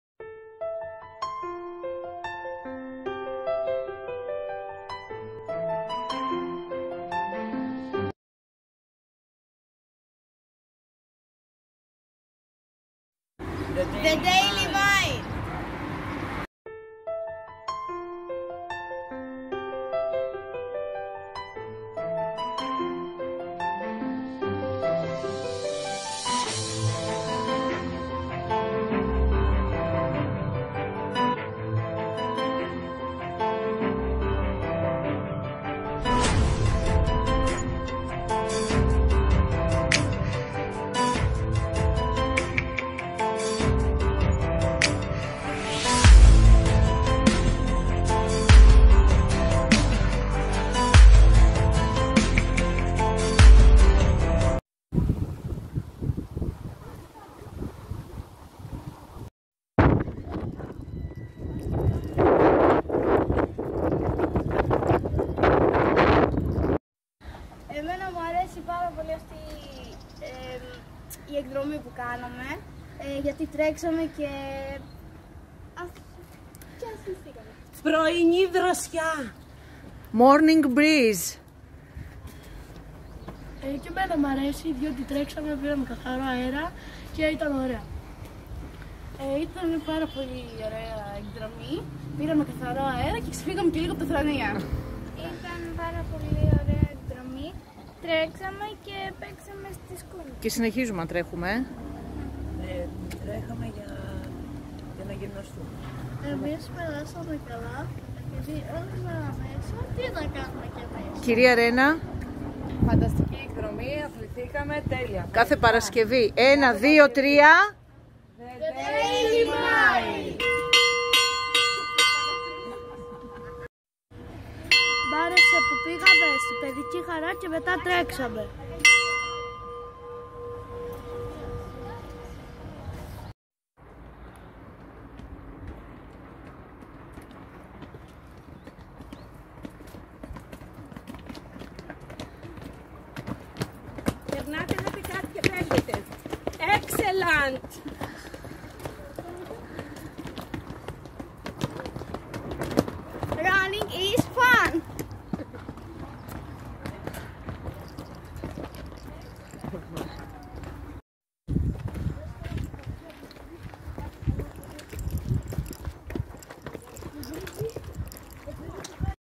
4trh LTTA - Rome - Pre mobil activ Part 4 - Running with us - The Daily Mile - Outdoor activ
Students 4rth premobility activity prior to the mobility in Rome - Training in the field - 2nd Primary School participate to the worldwide movement "The Daily Mile" for the dissemination of the Erasmus + project "Live mobile eat healthy catch quality"